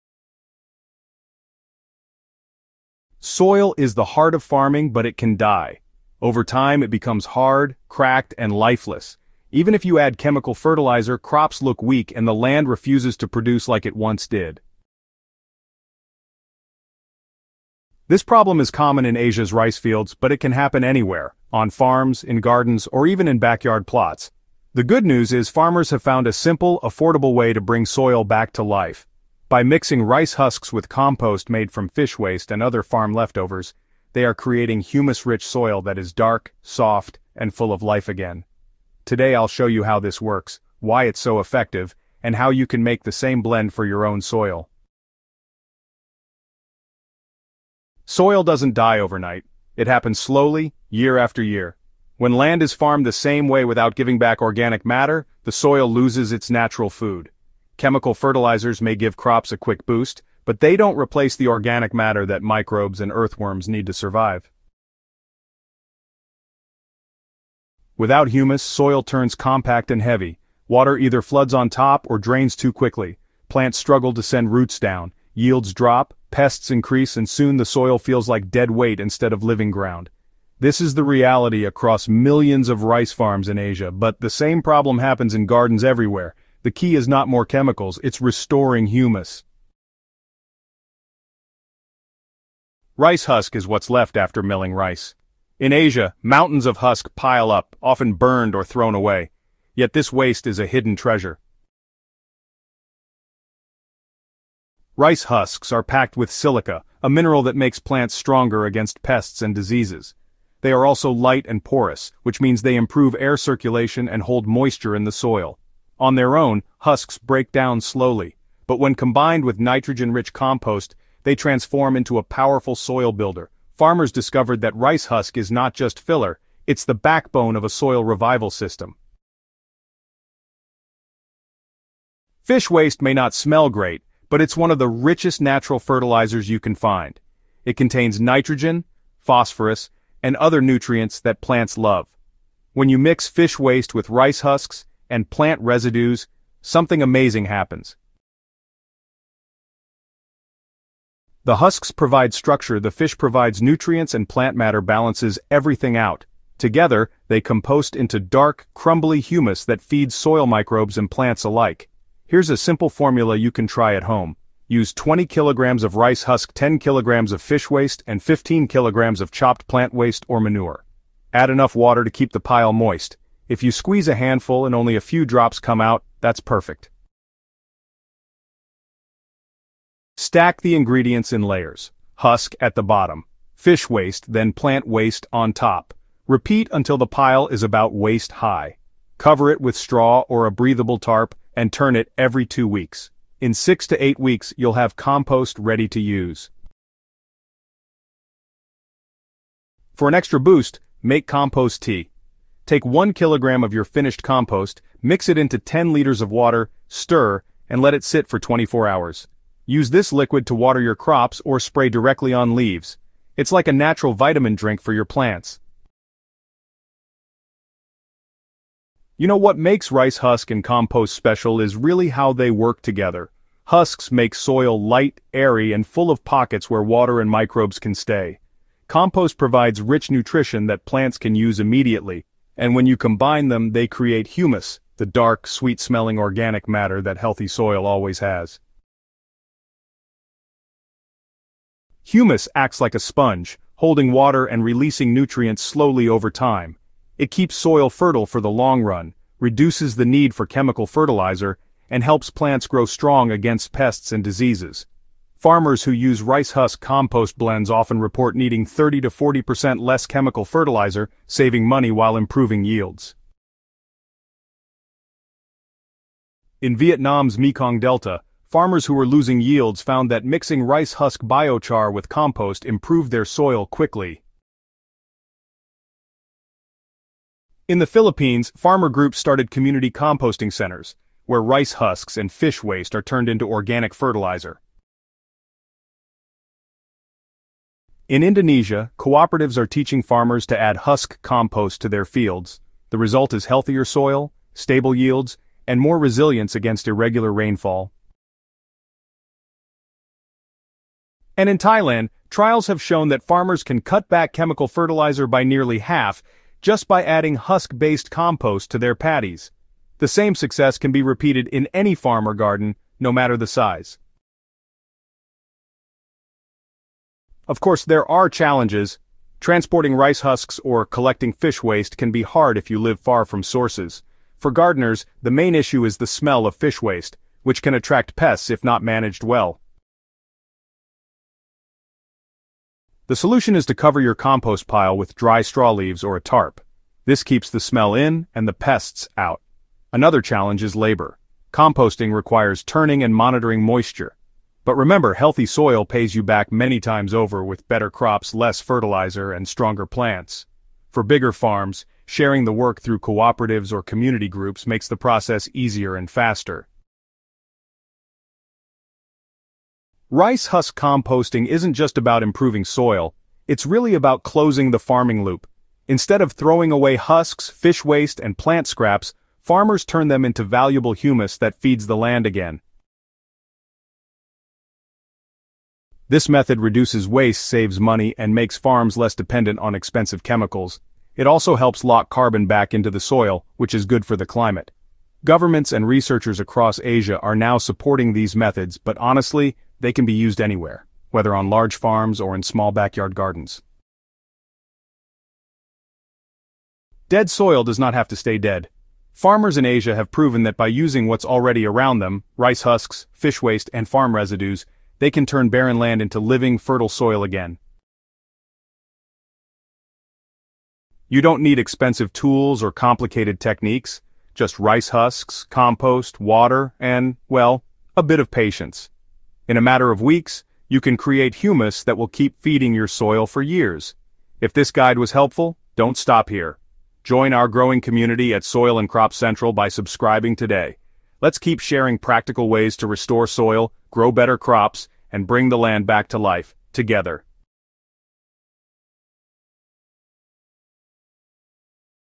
Turn Waste Into Rich Humus like Asians with Rice Husk and Fish Waste Compost
🌾 Is your soil DEAD and refusing to grow healthy crops? Don’t worry—you’re about to learn the proven secret farmers in Asia use to bring dead soil back to life. In this video, we reveal how rice husk compost blends with fish waste and farm residues are creating rich, black humus that transforms barren land into fertile, living soil again.

🔥 What you’ll discover inside:
✅ Why soils “die” and stop producing crops
✅ The hidden power of rice husks for improving soil
✅ Step-by-step composting formula with exact measurements
✅ How to make nutrient-rich compost tea for your plants
✅ Real farmer success stories from Asia you can copy
✅ How to reduce chemical fertilizers by up to 40%

💡 Whether you have a rice farm, vegetable plot, or backyard garden, this method works anywhere! It’s simple, cheap, and insanely effective.

👉 Watch till the end and learn how to:
🌱 Revive dead soil naturally
🌱 Create humus that feeds plants for years
🌱 Grow stronger, healthier crops with fewer chemicals

✨ Don’t miss this soil-saving trick!